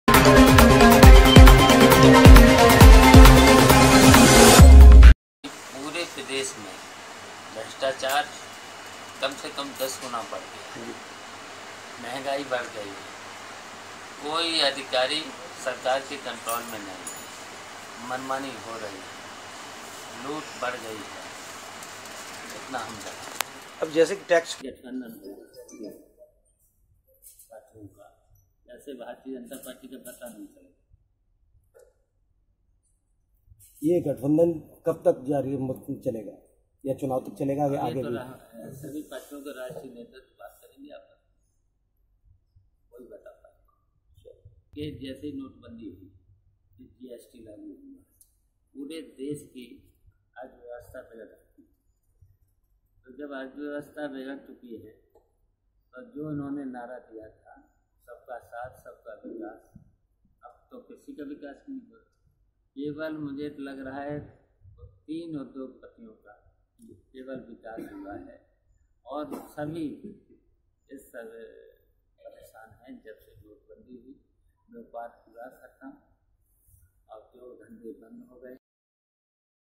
REPORTERS PAGE : इस सरकार में पूरे प्रदेश में भ्रस्टाचार 10 गुना बढ़ गया है- शिवपाल सिंह यादव
मैनपुरी पहुंचे शिवपाल सिंह यादव ने सरकार पर बड़ा हमला बोला है उन्होने कहा कि इस सरकार में पूरे प्रदेश में भ्रस्टाचार 10 गुना बढ़ गया है महंगाई बढ़ गयी है कोई भी अधिकारी इस सरकार के नियन्त्रण में नहीं है। वहीं आगामी चुनाव पर कहा कि-जैसे ही सभी पार्टियों का गठबन्धन होगा भारतीय जनता पार्टी का कहीं पता भी नहीं चलेगा वहीं उन्होंने कहा कि-नोटबंदी और जीएसटी लागू होते ही पूरे प्रदेश की अर्थव्यवस्था बिगड़ गयी थी सरकार ने नारा दिया था सबका साथ सबका विकास लेकिन केबल 3 व्यक्तियों का ही विकास हुआ है